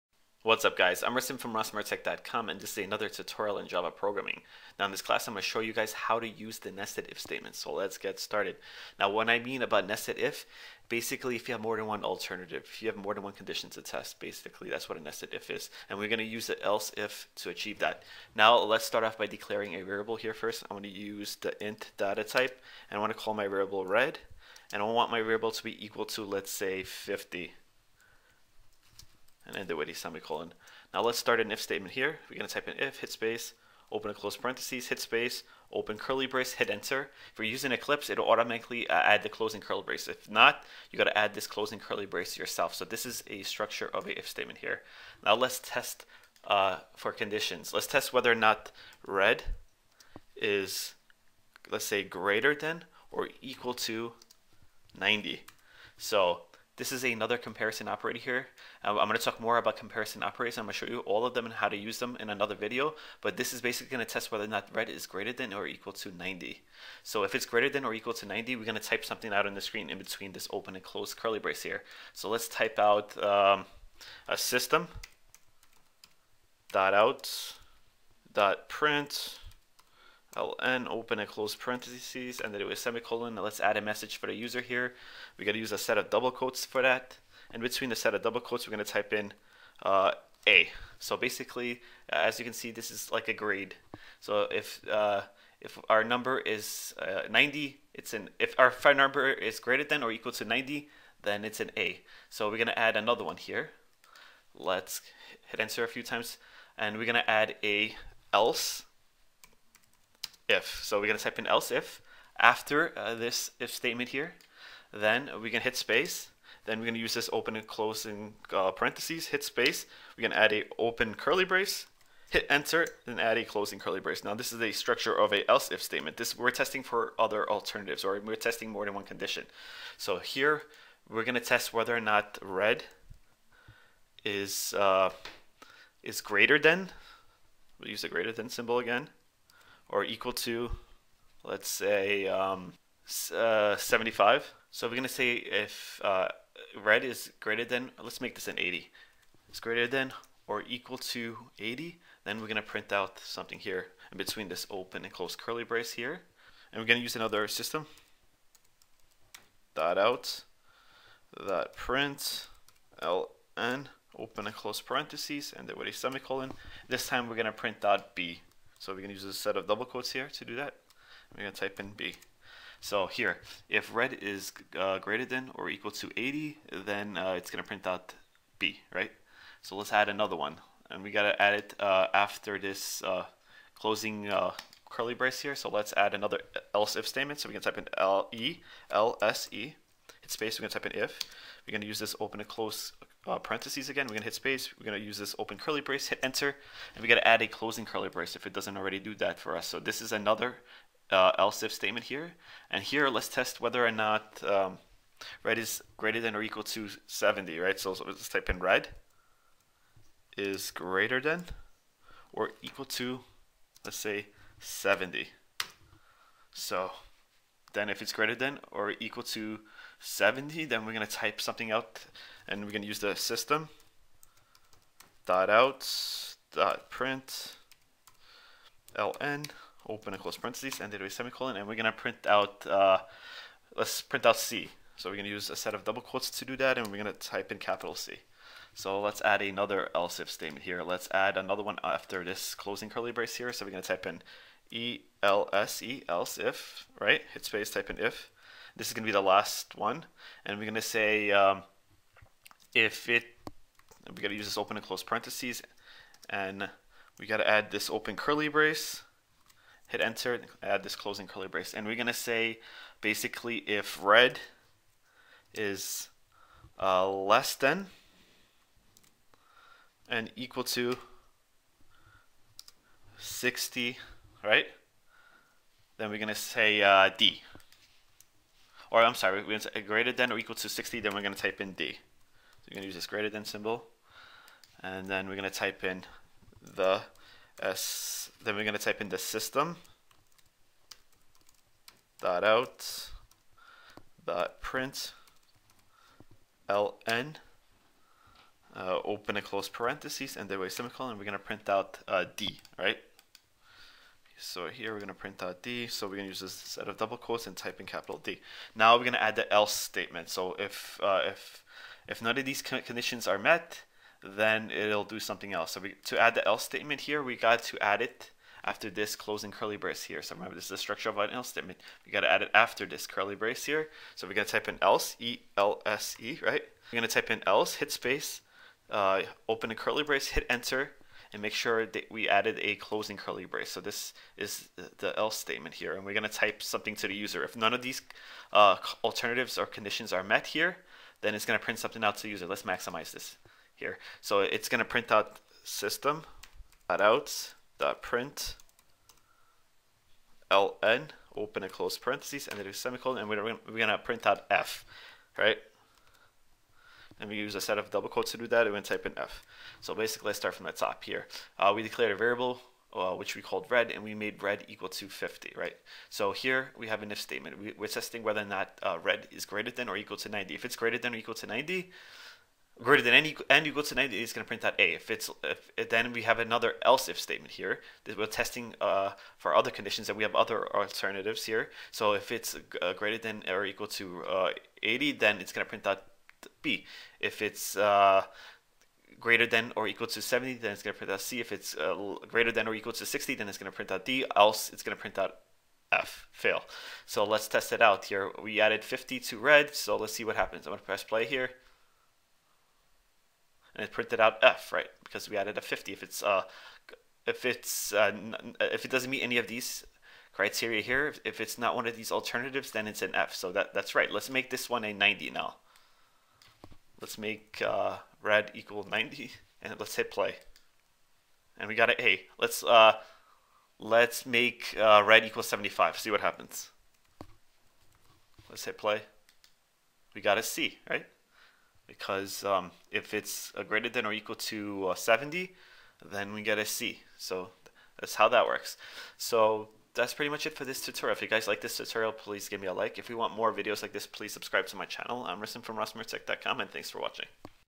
Java Programming Tutorials - 17 - Nested If Statement
In this tutorial I will show you how to use the Nested If Statement.